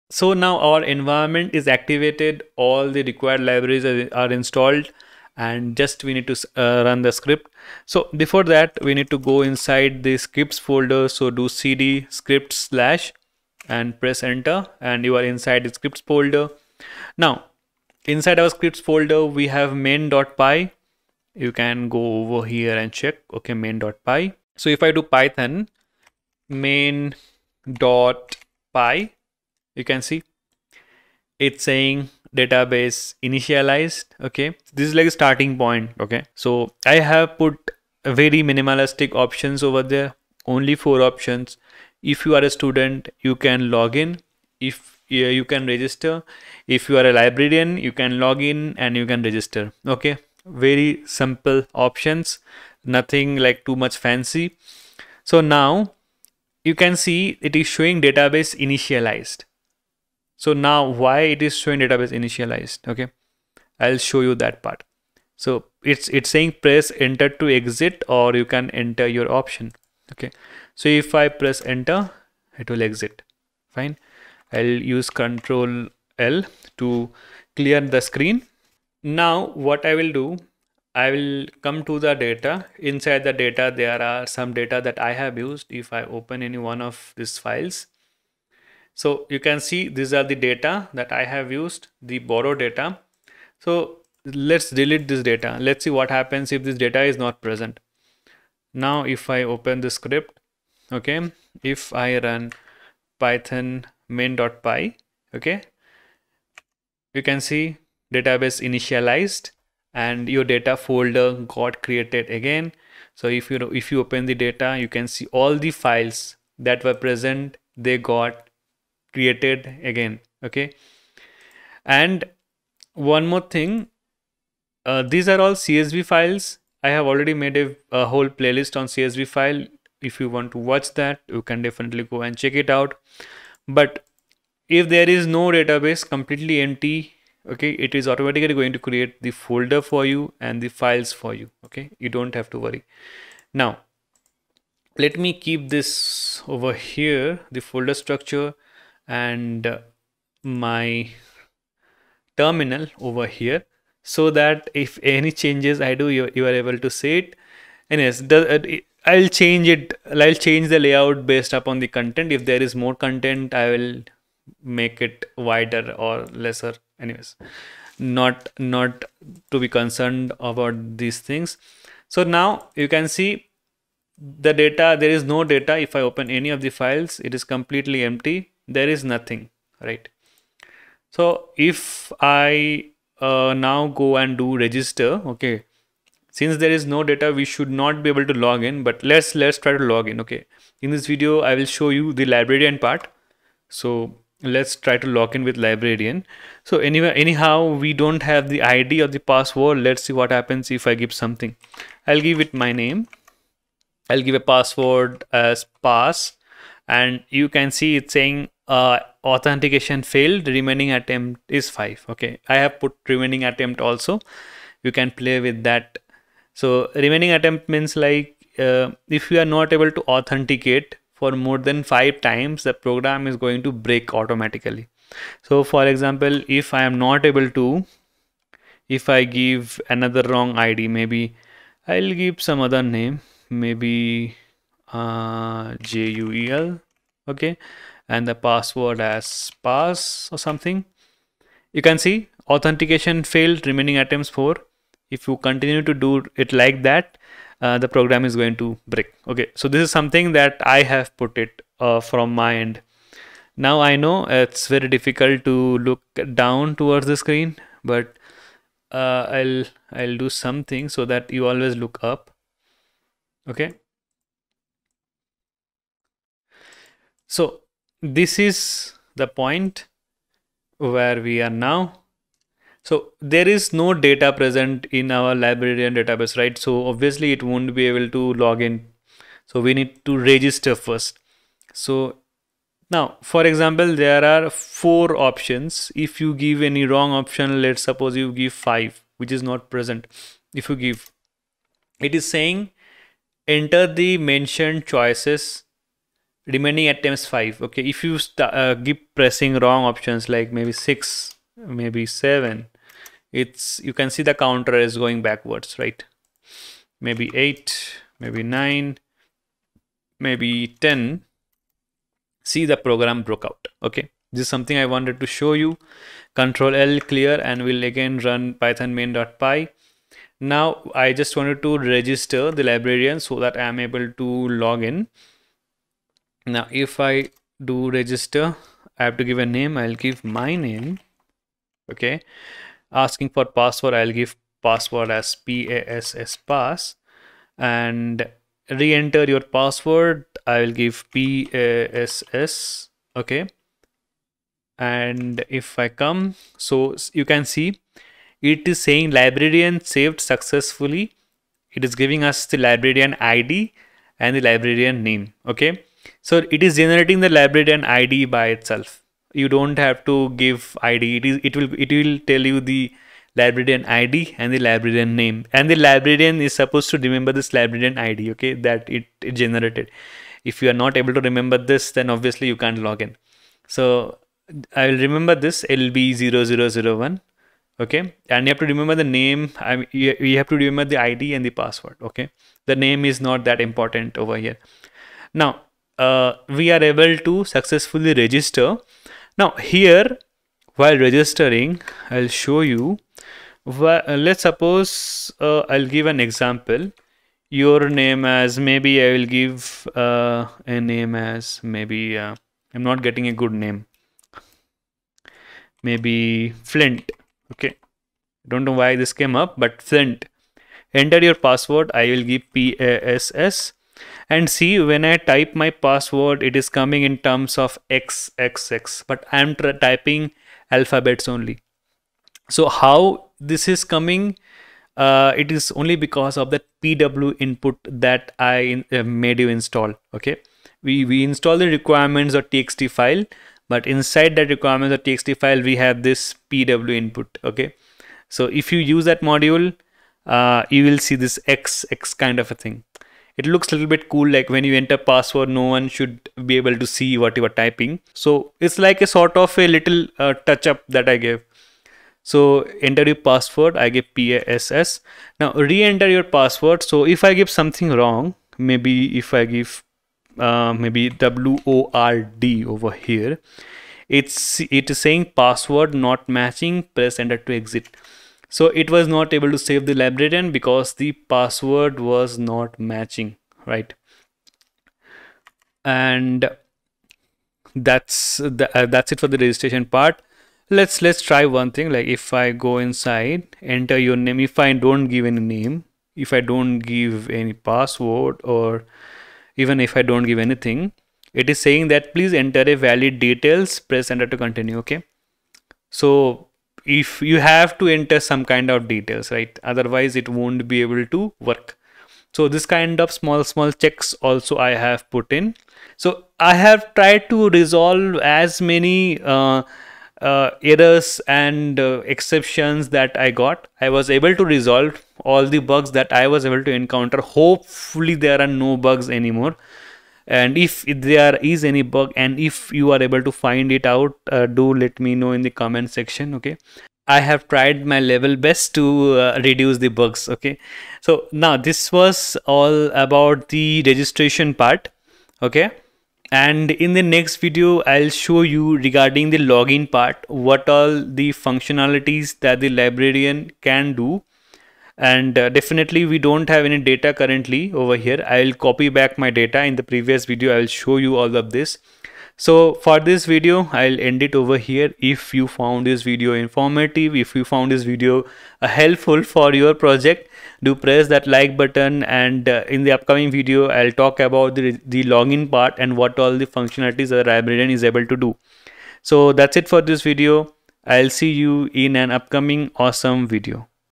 Librarian Registration | Library Management System | Part 3
This project is a classic Library Management System. The application is created using Python Programming Language. This application uses Command Line Interface.

► Download Links

► Other Links

► About Me
▸Biswajit Basak
▸B. Tech in Computer Science
▸Batch of 2019